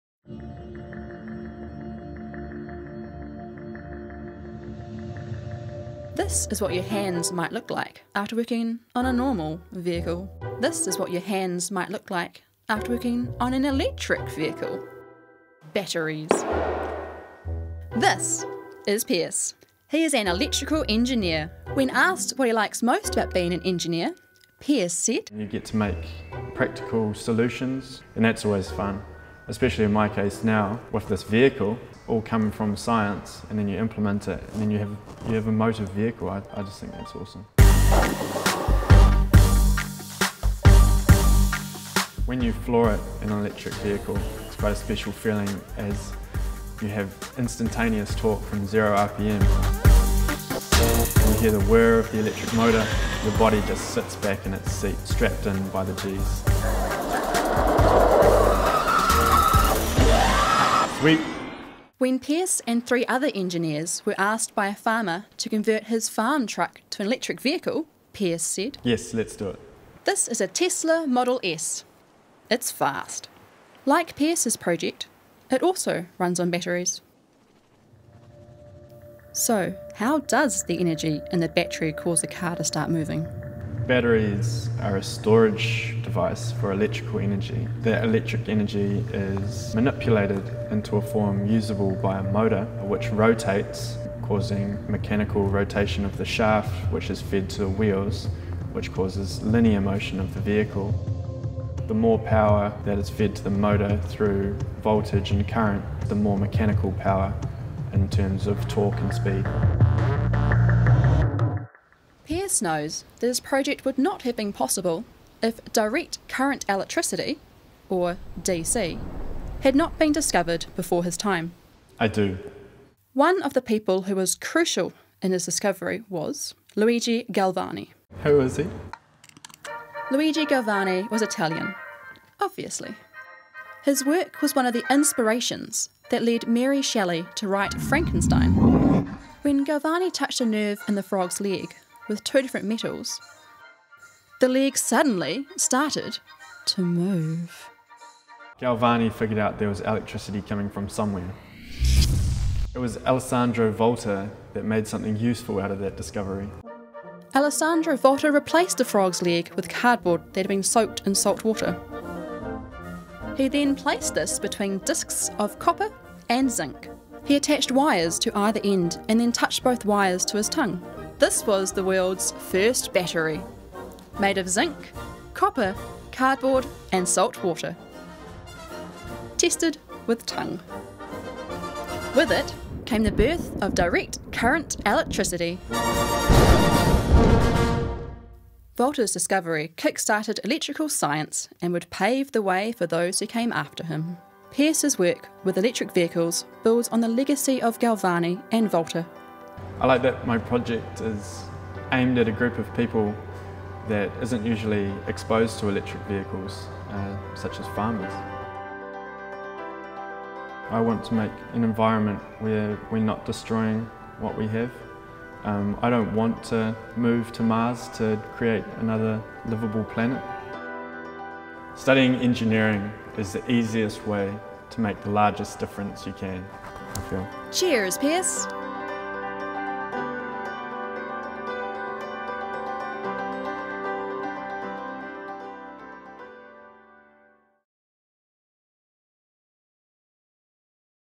Electrical Vehicles and DC electricity
Electrical Engineer Pierce Hennessey shows us how Direct Current electricity from energy stored in batteries is converted to mechanical power to drive fast electric vehicles.